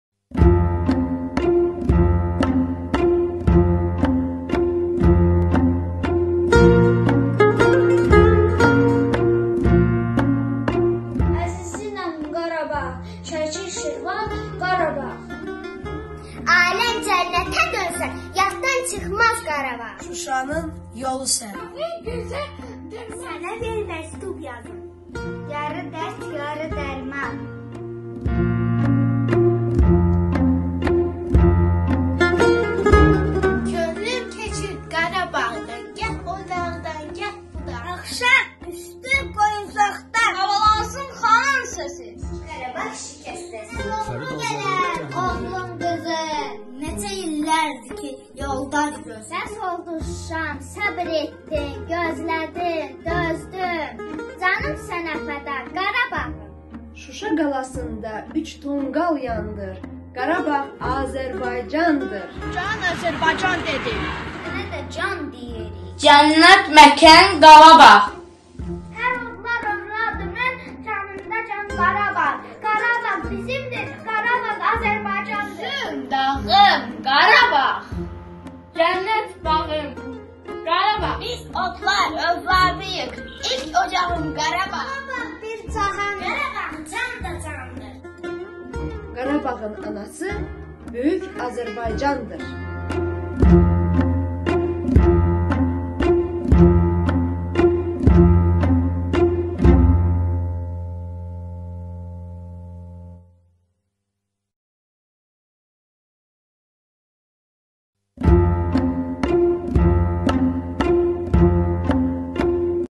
4e sinif şagirdləri 9 Noyabr Bayraq Gününü belə qeyd etdilər
Zaur Məmmədov adına 171 nömrəli tam orta məktəbin 4e sinif şagirdləri 9 Noyabr Bayraq Gününü belə qeyd etdilər